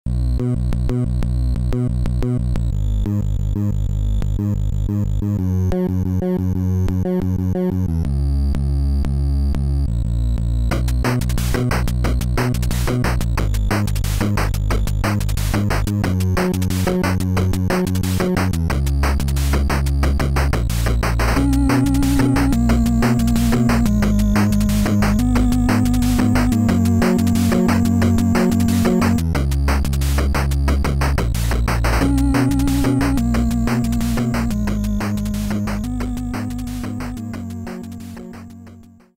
Custom 2A03 Music Driver Test - Currently in Development
Yeah. I really am programming my own music driver to showcase this one (1) trick. I definitely have my priorities straight.